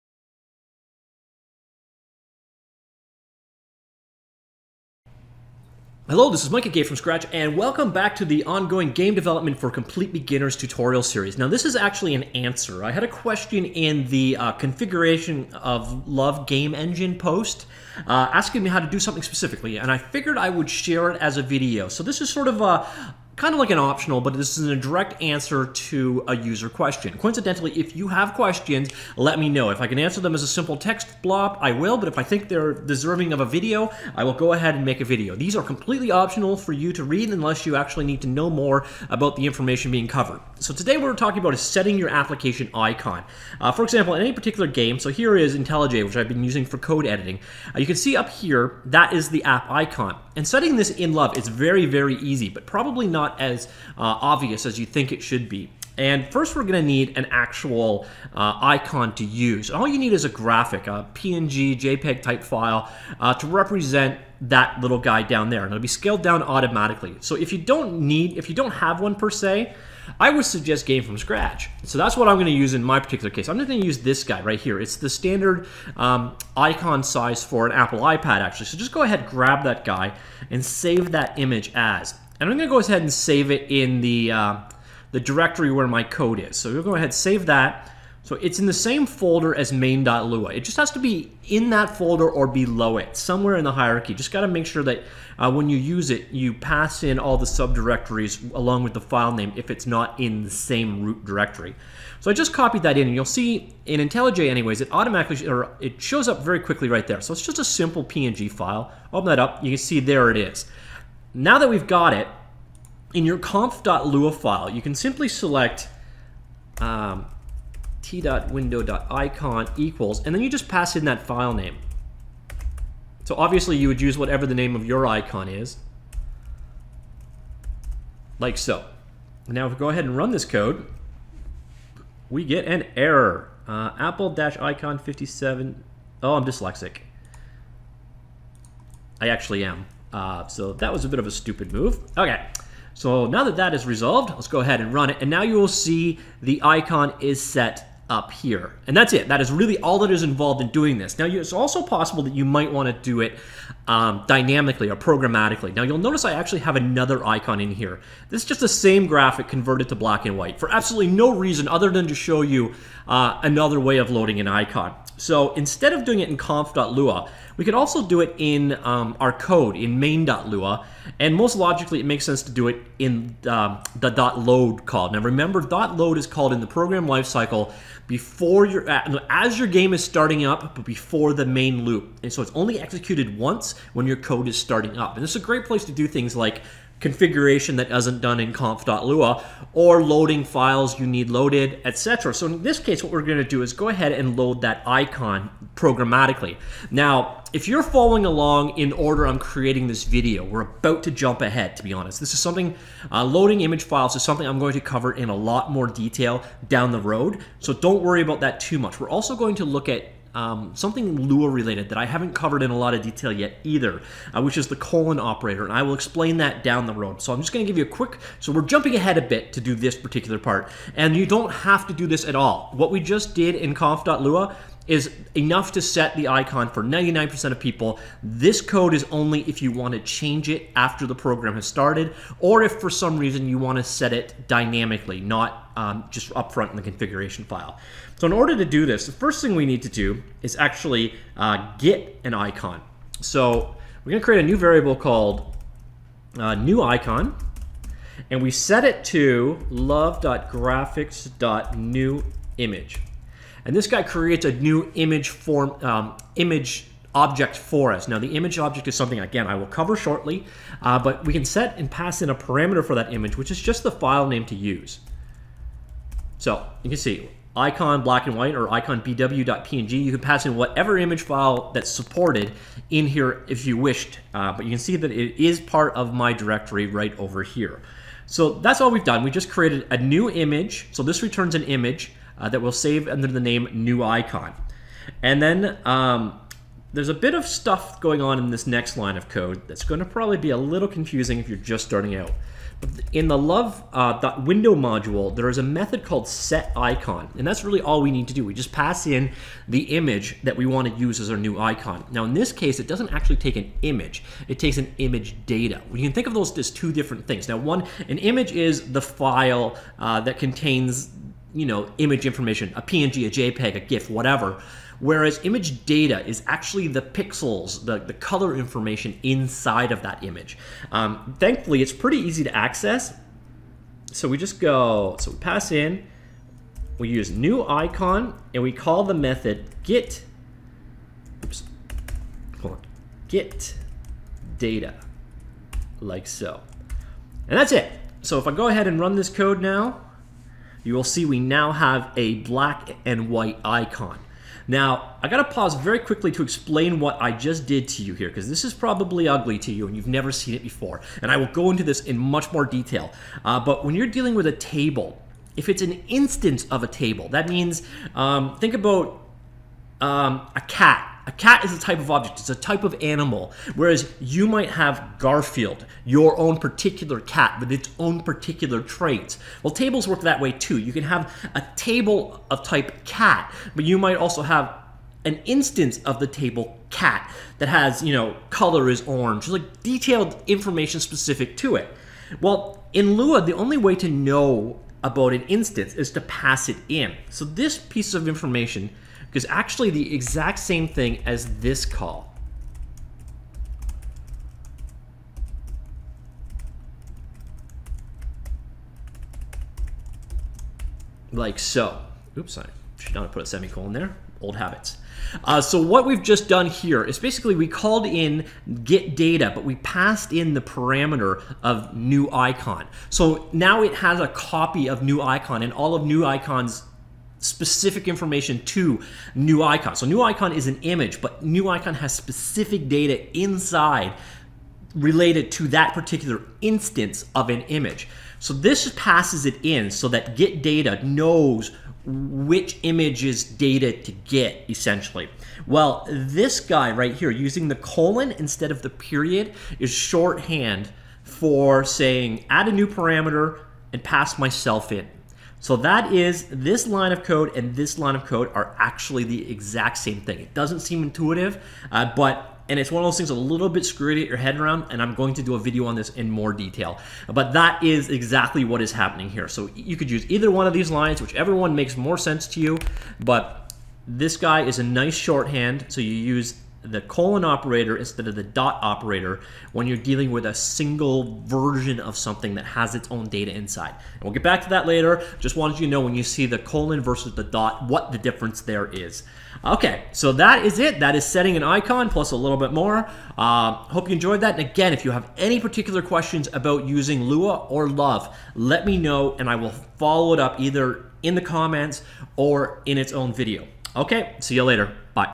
QA - Setting an App Icon in Love -- Game Development For Complete Beginners Tutorial Series
This video illustrates how to set the application icon in a Love application, both in Conf.lua and in code.  This was in direct response to a question asked during the Configuring Lua Tutorial.

The video also covers a bit about the . vs : operator in Lua as well as dealing with loading Image files, but both of those topics will be covered in more detail in the main tutorial.
If you have specific questions, please let me know!